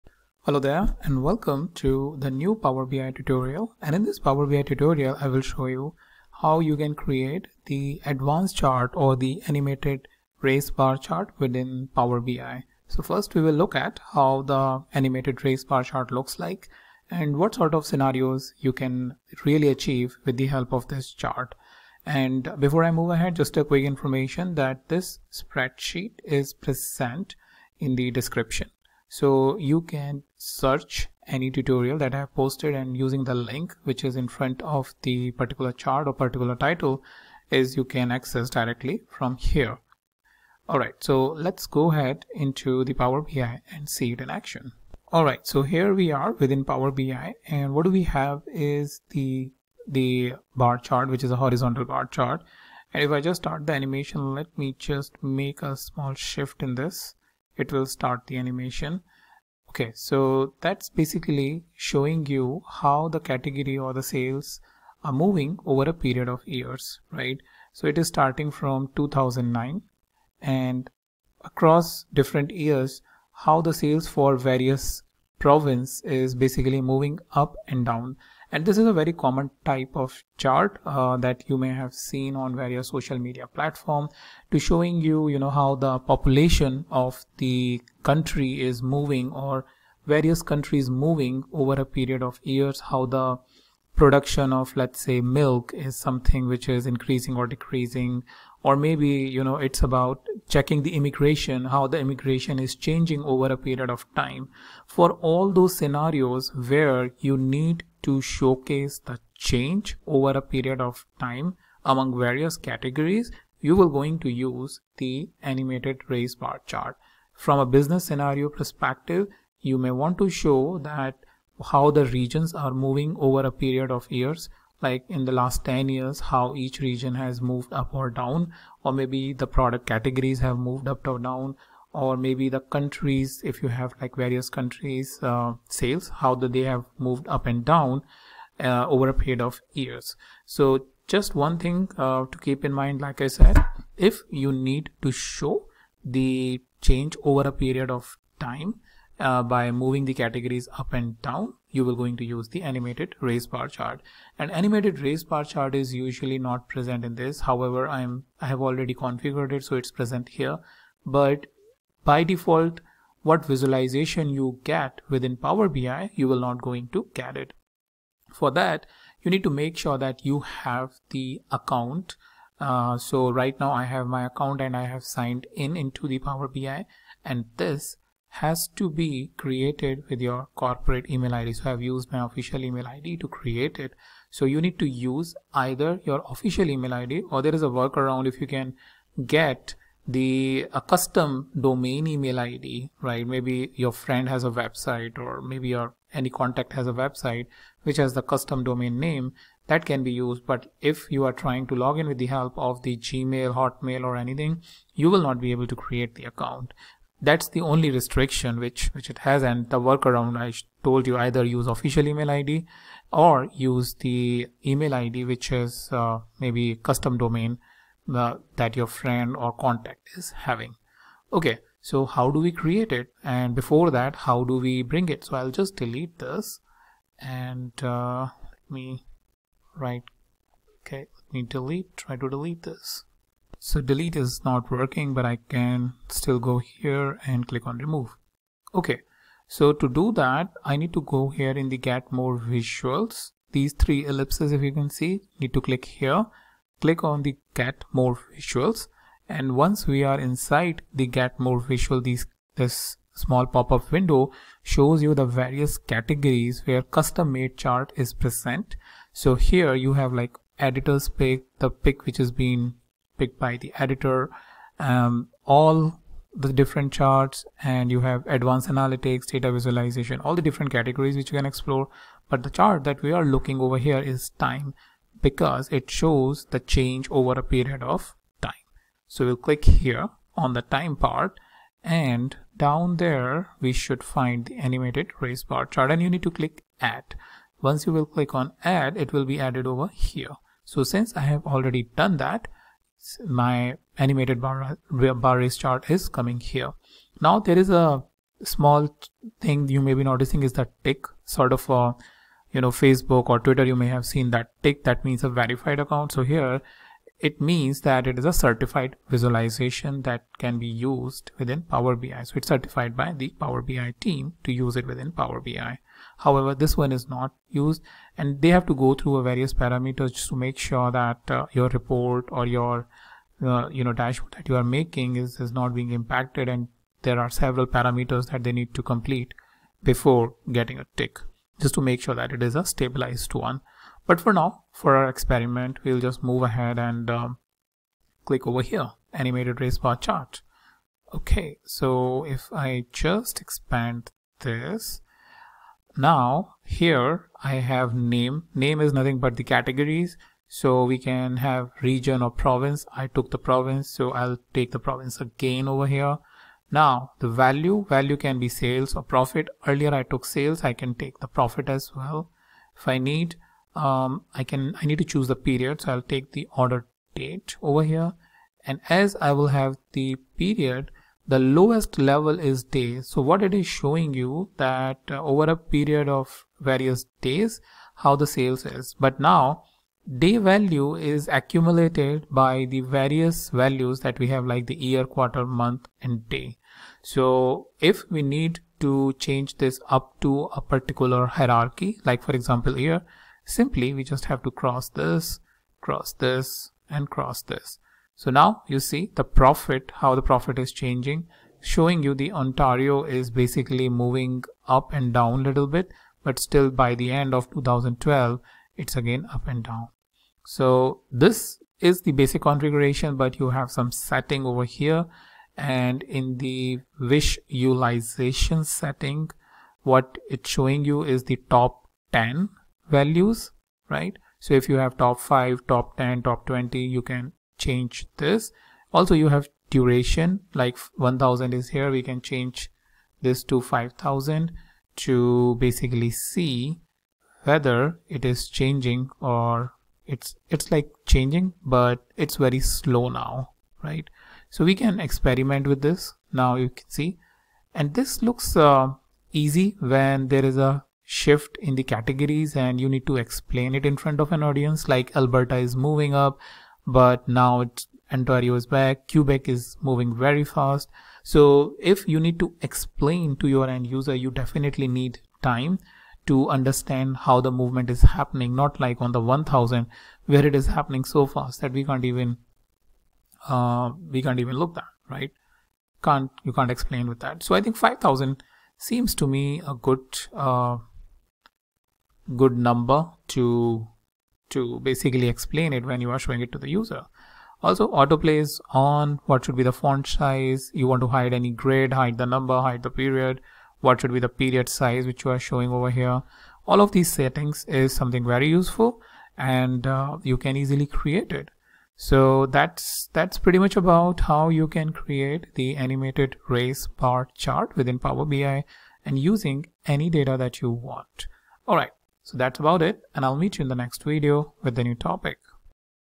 Power BI Animated Bar Race Chart Tutorial
Power BI tutorial for beginners on how to create animated bar race chart in power bi and customize it to show the most impactful information in right way.